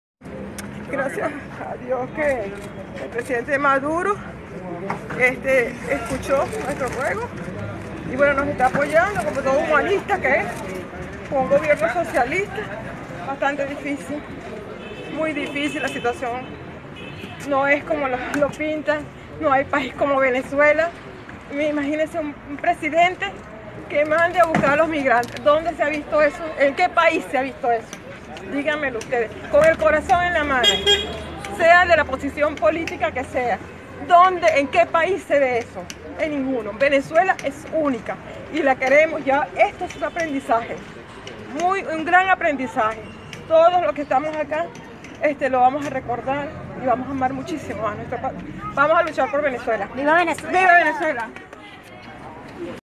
Venezolana, en Lima: Maduro es un humanista y el único que manda a buscar a los migrantes
ND / 8 sep 2018.- Un grupo de venezolanos se prepara para regresar al país en un viaje organizado y pagado por el Gobierno nacional. La cancillería precisó que se trata del plan "Vuelta A La Patria, ideado por el Presidente obrero Nicolás Maduro".

Una mujer, Isbelis Herrera, dio estas declaraciones mientras se encontraba al lado del autobús que la llevaría al aeropuerto de Lima: "Gracias al Dios que el presidente Maduro escuchó nuestro ruego y nos está apoyando como todo humanista que es, con un gobierno socialista... ha sido bastante difícil. Muy difícil la situación. No es como lo pintan. No hay país como Venezuela. Imagínese un presidente que manda a buscar a los migrantes. ¿Dónde se ha visto eso? ¿En qué país se ha visto eso? Díganmelo ustedes con el corazón en la mano, sea de la posición política que sea. ¿En qué país se ve eso? En ninguno. Venezuela es única y la queremos.

Luego remató: "Esto es un aprendizaje! Un gran aprendizaje. Todos los que estamos acá lo vamos a recordar... vamos a luchar por Venezuela".